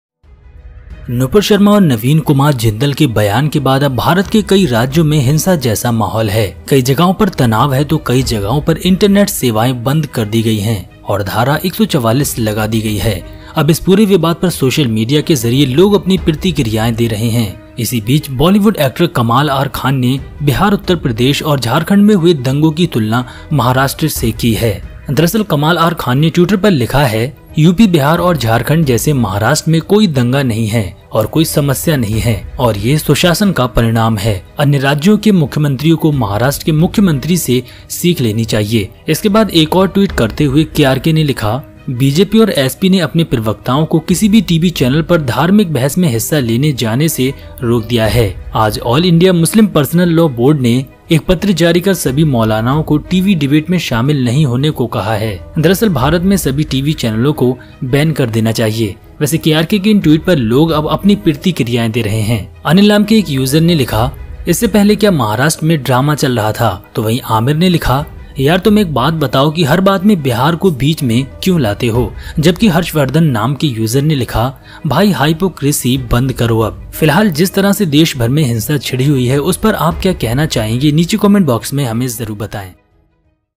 KRK tweet on Yogi Government | Kamaal Rashid Khan | Nupur Sharma Video | Prophet Muhammad Insult
After the statements of Nupur Sharma and Naveen Kumar Jindal, now there is an atmosphere like violence in many states of India. There is tension in many places, internet services have been stopped in many places and section 144 has been imposed. Now people are giving their reactions on this entire controversy through social media. Meanwhile, Bollywood actor Kamal R Khan has compared the riots in Bihar, Uttar Pradesh and Jharkhand with Maharashtra.

In fact, Kamal R Khan wrote on Twitter that there is no riot in Maharashtra like UP, Bihar and Jharkhand and there is no problem and it is the result of good governance. Chief Ministers of other states should learn from the Chief Minister of Maharashtra. After this, in another tweet, KRK wrote, 'BJP and SP have stopped their spokespersons from participating in religious debates on any TV channel. Today All India Muslim Personal Law Board issued a letter asking all the Maulanas not to participate in TV debates. Actually all TV channels should be banned in India.

By the way, people are now giving their reactions on these tweets of krk. A user named Anil wrote, 'Earlier, was there drama going on in Maharashtra, while a user named Aamir wrote, 'Man, tell me why do you bring Bihar in the middle of everything?' While a user named Harshvardhan wrote. Wrote, Brother Stop Hypocrisy Now.